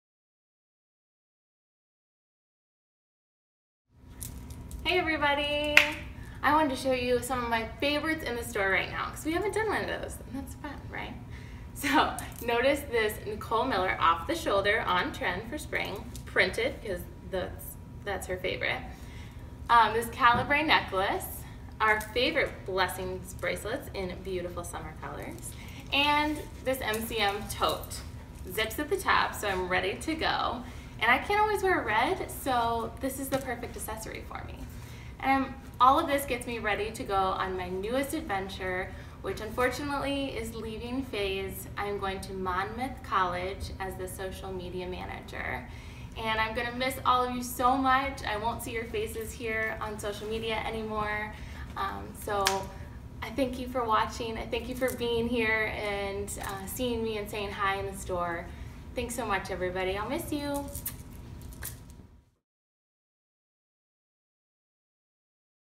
Faye's Thursday Trends: Farewell Emilee
Faye's Thursday Trends, Episode 47, Farewell Emilee
(Emilee shares her Faye's favorite summer pieces!)